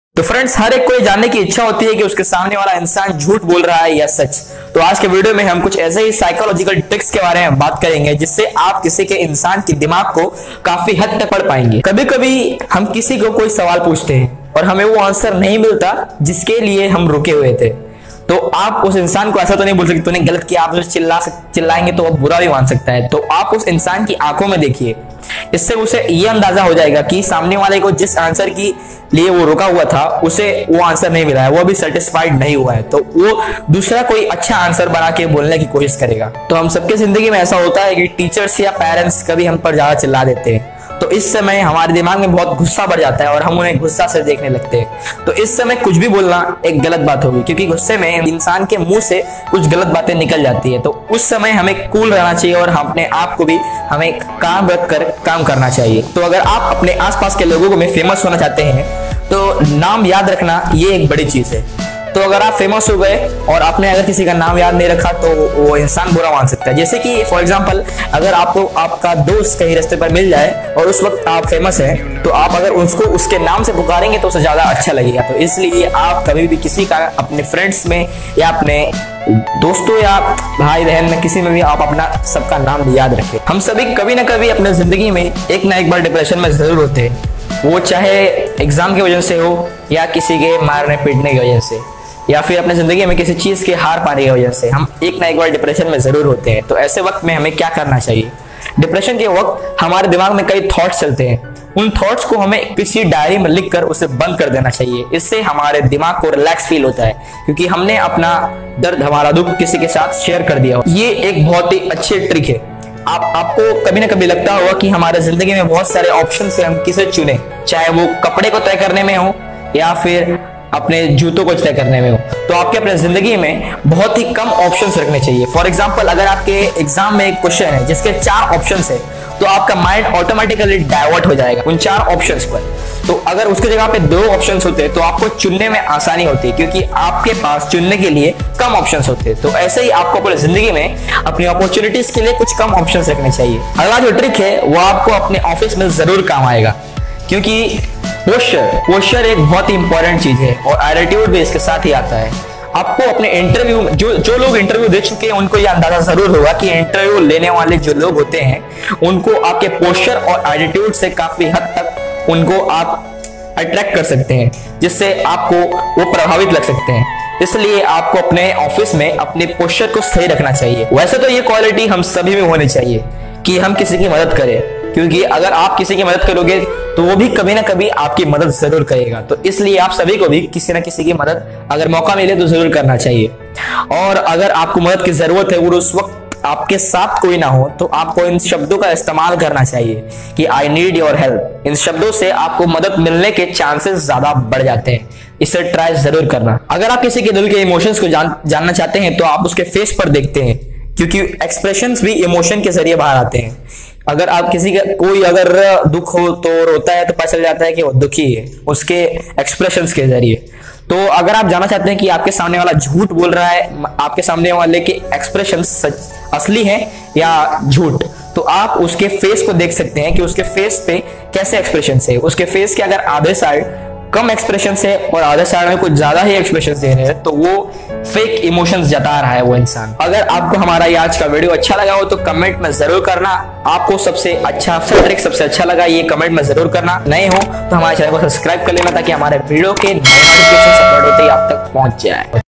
Amazing Psychological Facts || Psychological Tricks To Influence Other People
Understanding of Psychology comes with its own perks. Once you understand how and why humans behave the way they do, you can navigate through awkward situations, get a raise, make more sales, help others, be more successful, make more money, influence people, improve your quality of life and more. By using these Psychological hacks you'll be able to not only communicate well and make better social connections but you'll also become happier, increase your productivity, improve your health and learn/study faster. We have discussed 7 simple but shocking psychological facts and tricks that will increase your knowledge of psychology and human behavior.